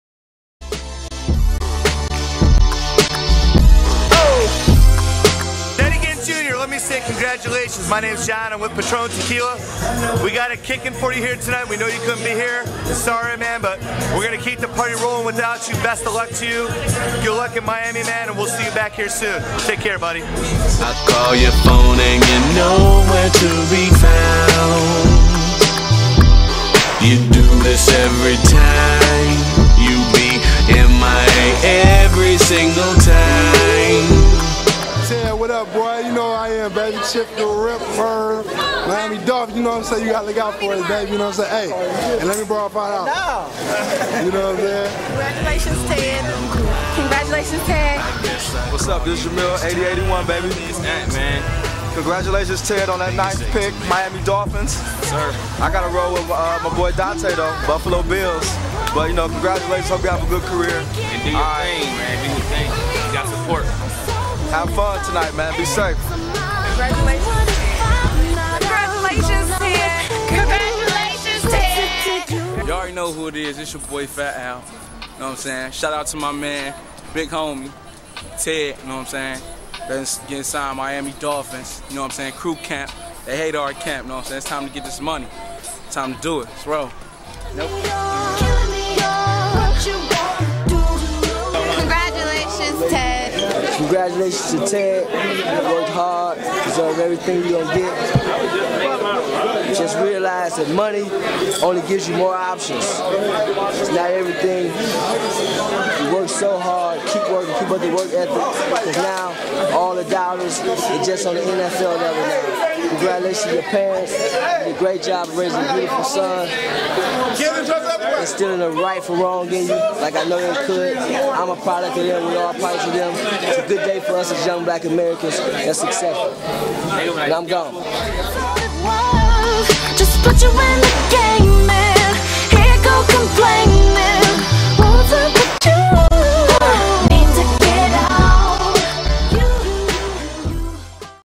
Ted Ginn Jr Draft Party
words from event sponsor Patron, Chip Tha Ripper, Al Fatz, and 8081.